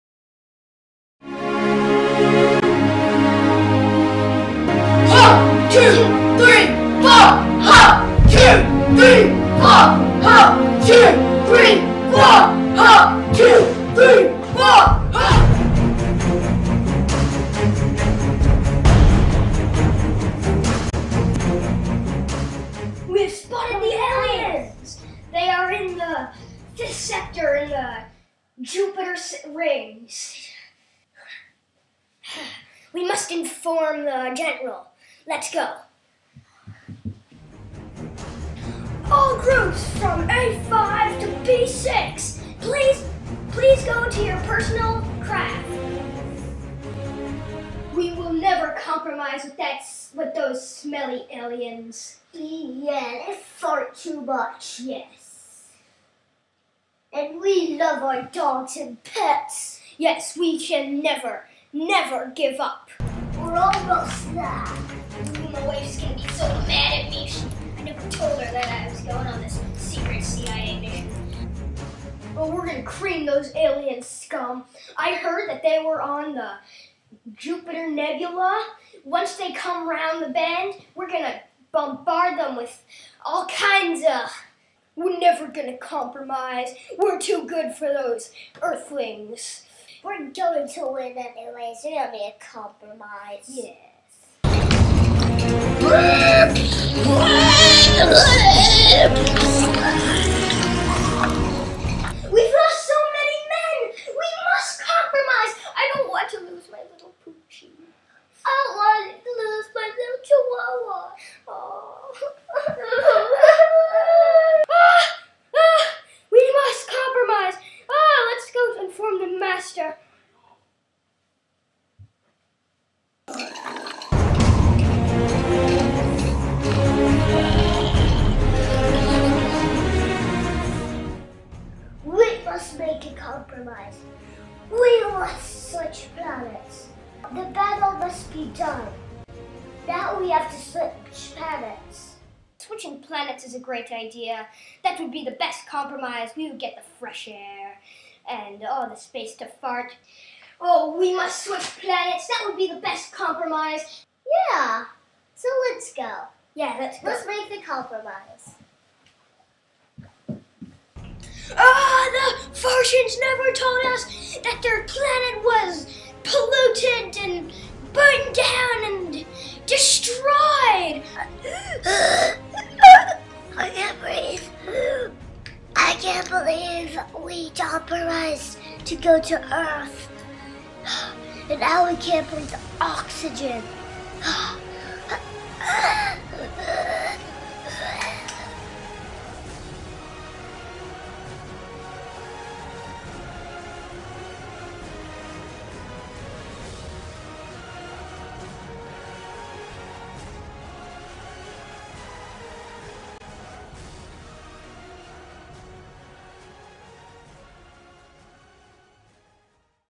FART MARS II (ep. 2)
Aliens and humanoids are pushed to make a dangerous compromise.